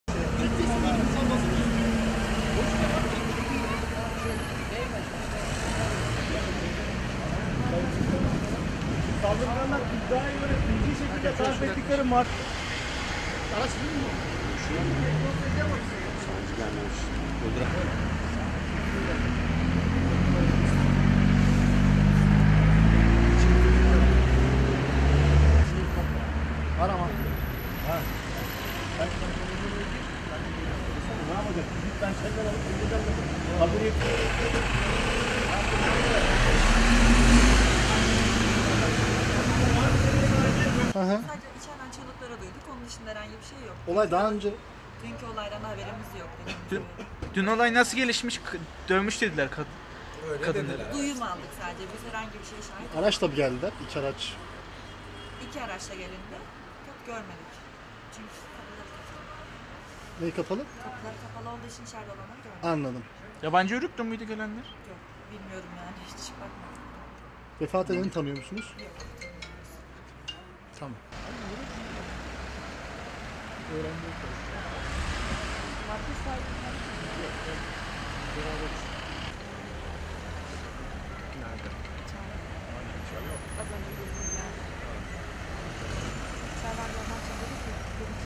Ankara’da markete kaza ; 1 ölü, 2 yaralı
Ankara’da 10 kişilik bir grup, husumetli oldukları şahsın marketini bastı. Çıkan olayda 1 kişi hayatını kaybetti, 2 kişi yaralandı. Olayla ilgili 6 kişi gözaltına alındı.